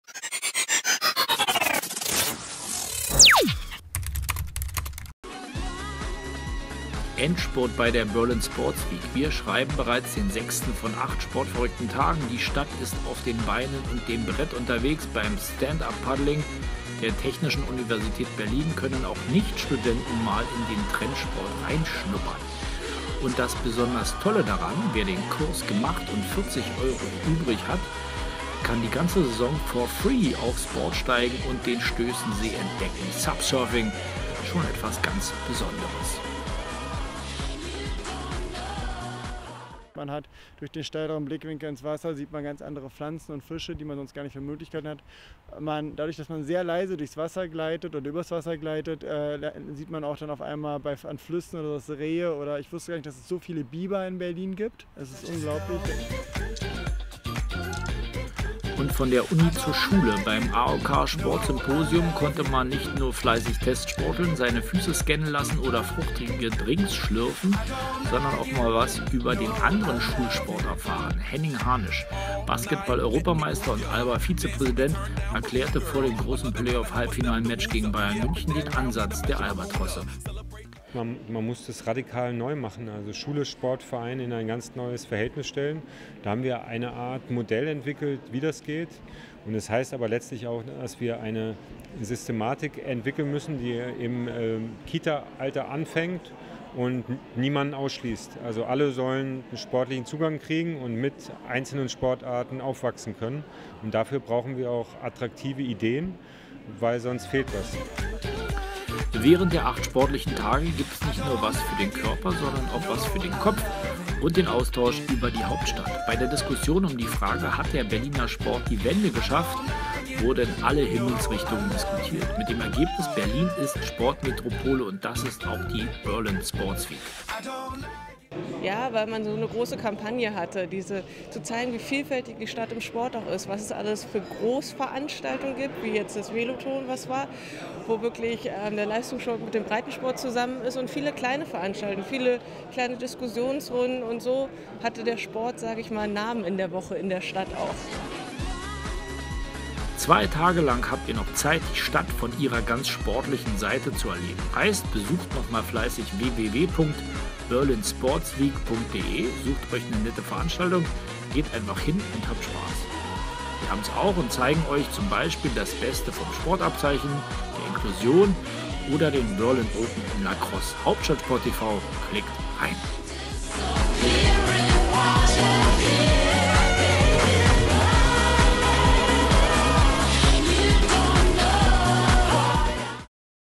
Berlin Sports Week 2015 | Tag 6 - Endspurt!
Endspurt bei der Berlin Sports Week! Wir schreiben bereits den sechsten von acht sportverrückten Tagen.